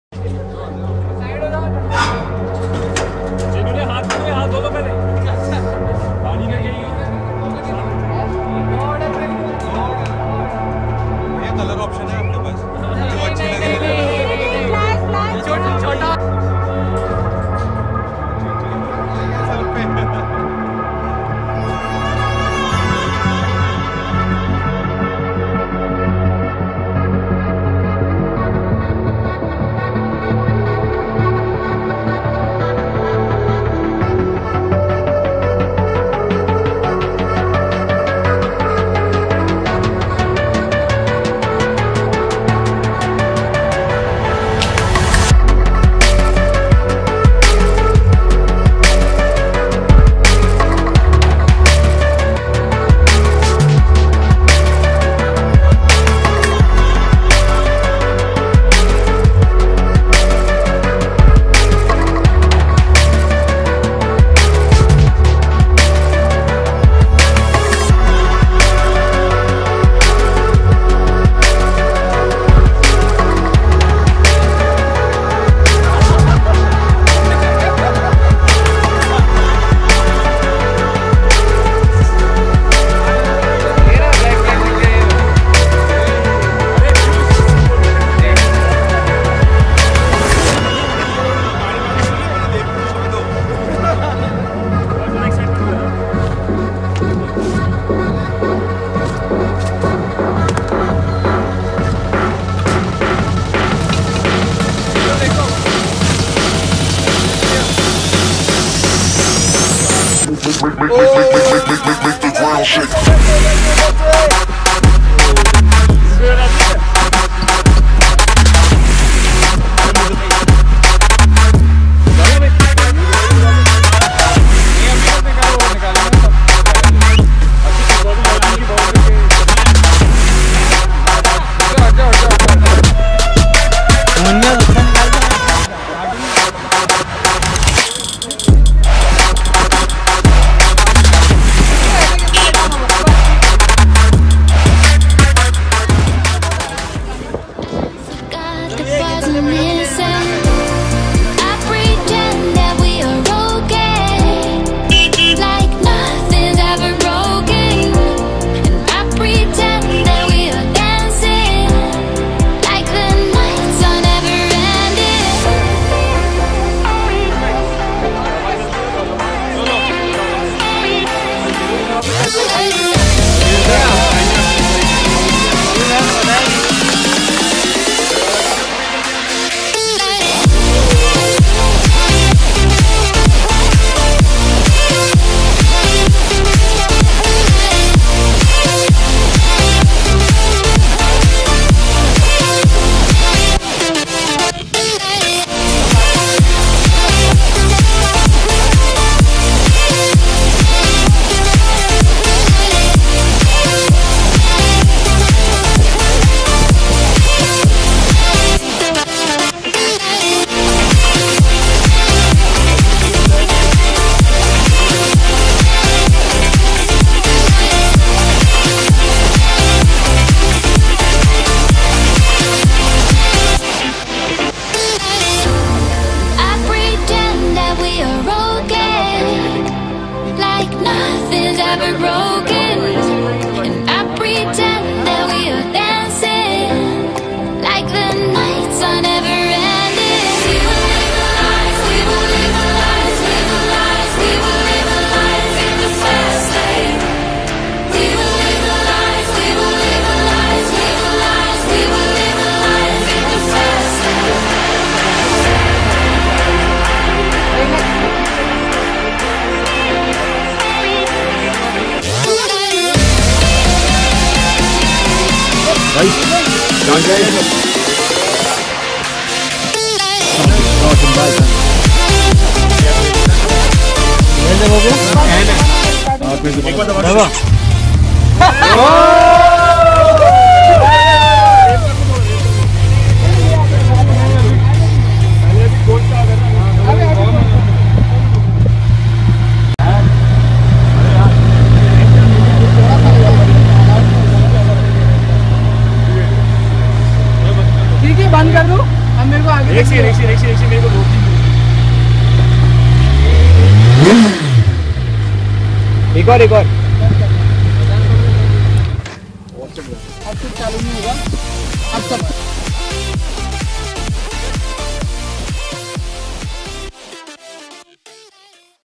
Kawasaki Ninja 1000 Unboxing LIMITED EDITION | Exhaust note | Indore | JBiker 83
2019 kawasaki ninja 1000 unboxing